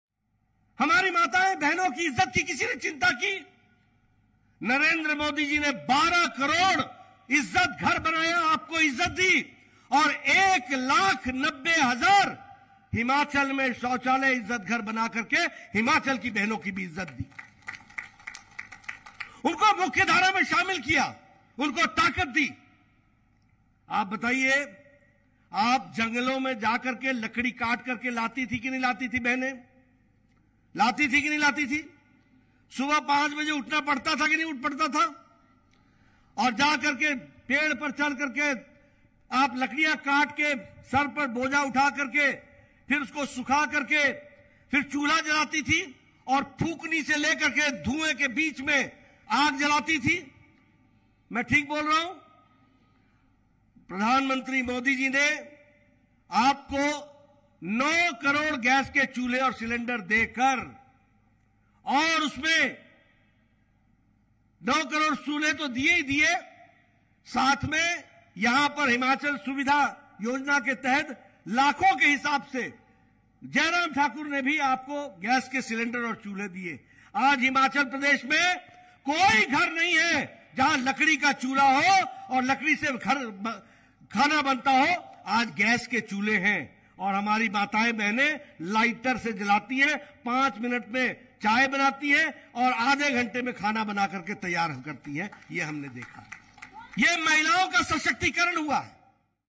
भाजपा की डबल इंजन सरकार ने हिमाचल में शौचालय बना कर माताओं-बहनों को सम्मान दिया।
'उज्जवला योजना' के माध्यम से महिलाओं की रसोई तक गैस सिलेंडर पहुंचाया।

'नल से जल' योजना के माध्यम से घर-घर में जल पहुंचाया।

- श्री @JPNadda